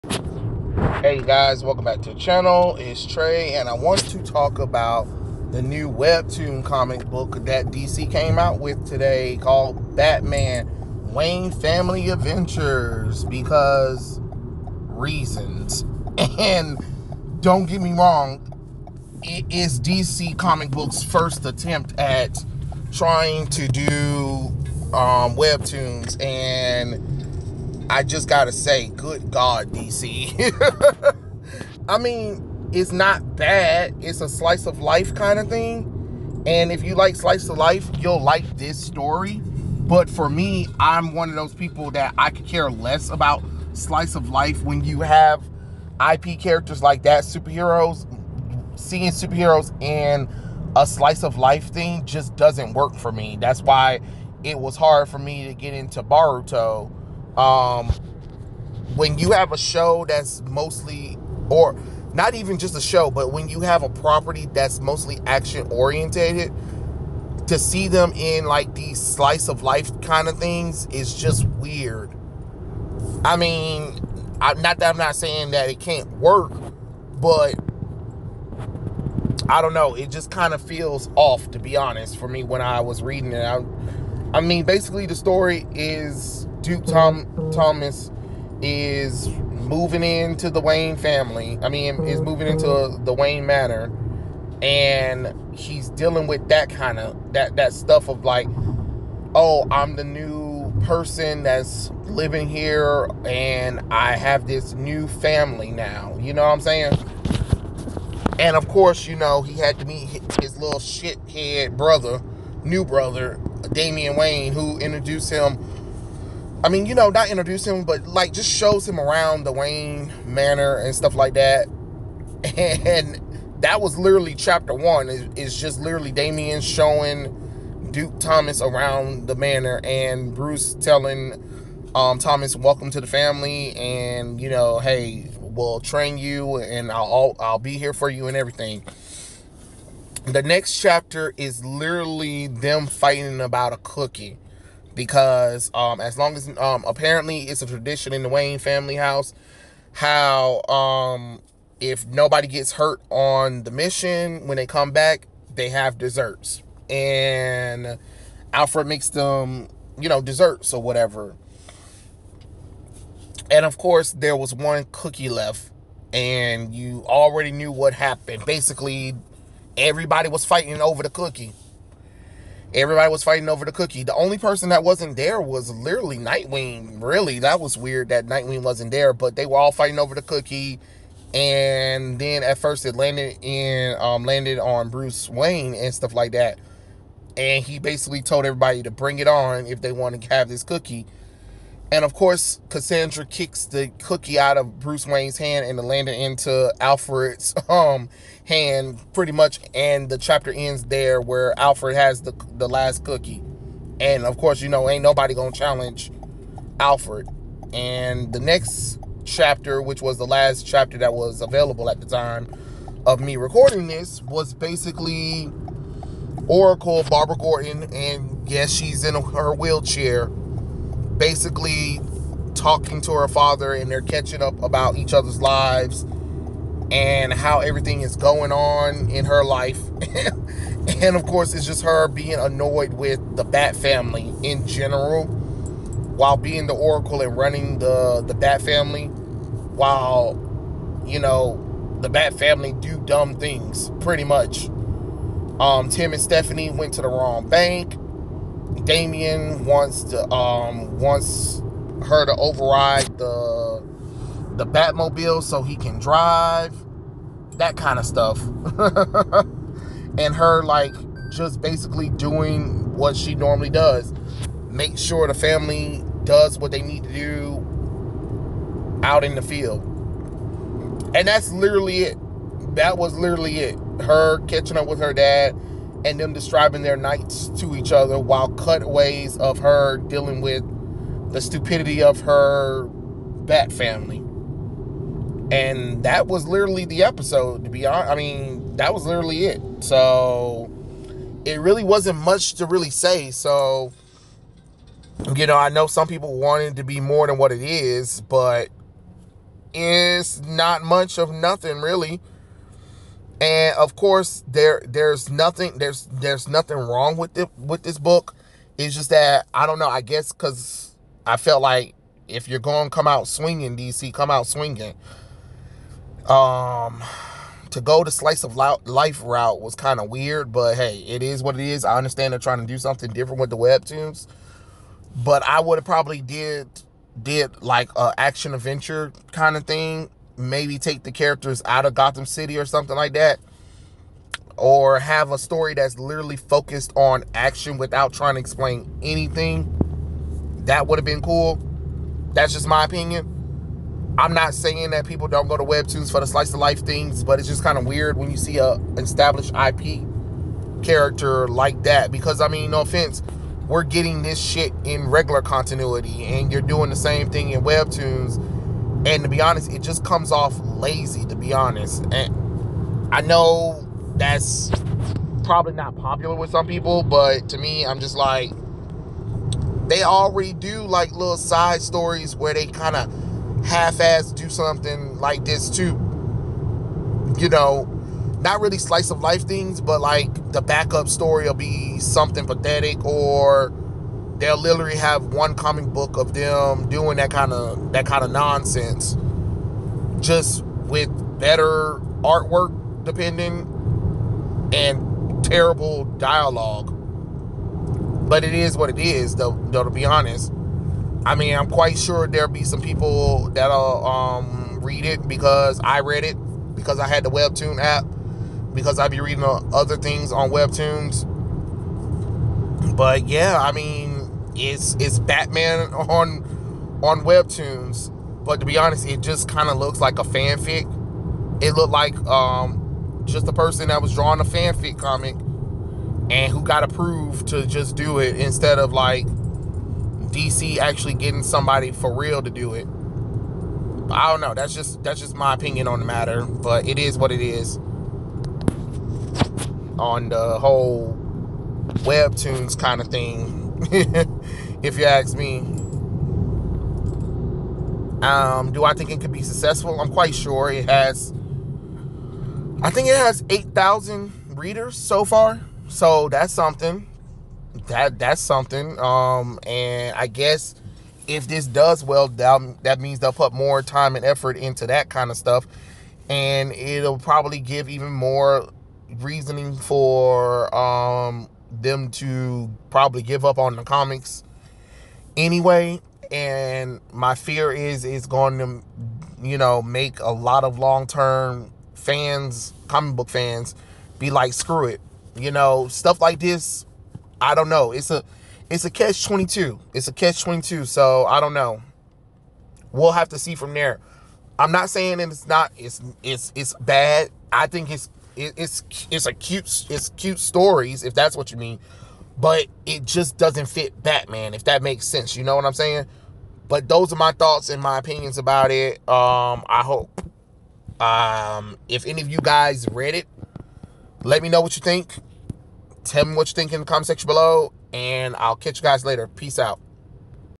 DC Comics First Webtoon... it's Something...?
So DC Comics was for real about this Webtoons. There first one of course start with Batman sadly. But here it is Webtoons Comics now!!